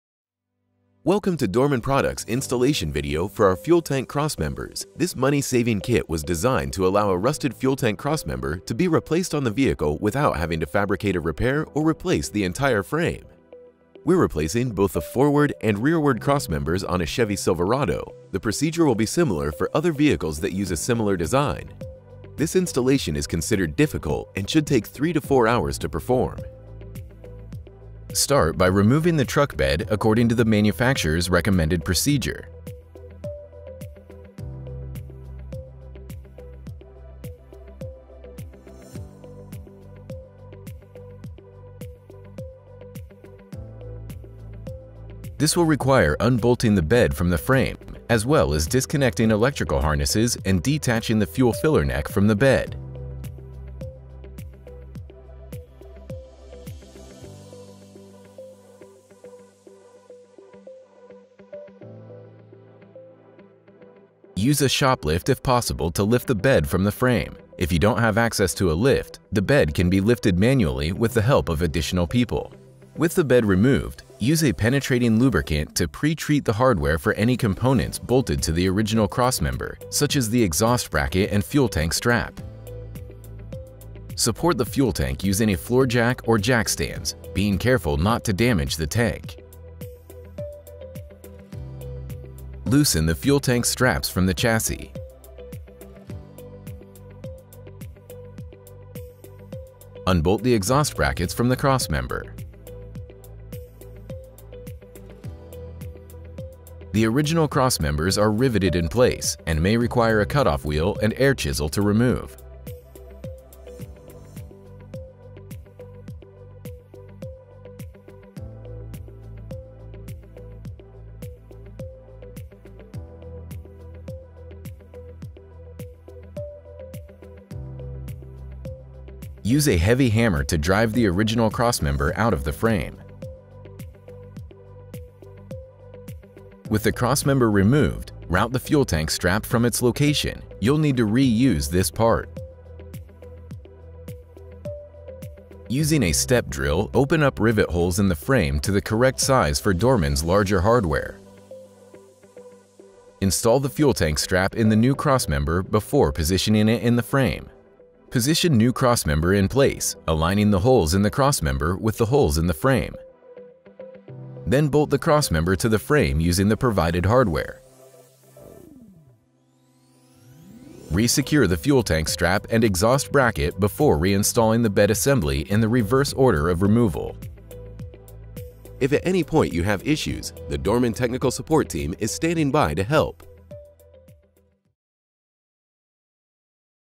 Fuel Tank Crossmember Kit Installation by Dorman Products
Fuel tank loose and no longer supported? This fuel tank crossmember replaces an original equipment crossmember damaged by corrosion. Made of durable steel, it is finished in weldable primer for a clean, reliable repair.

In this video, we show you how to properly and safely install this cost-saving new solution. This is a difficult repair, so please follow all safety procedures and get additional help if needed. If at any point you have any questions, the Dorman Technical Support team is standing by to help

Find the right fuel tank crossmember solution for your vehicle at DormanProducts.com.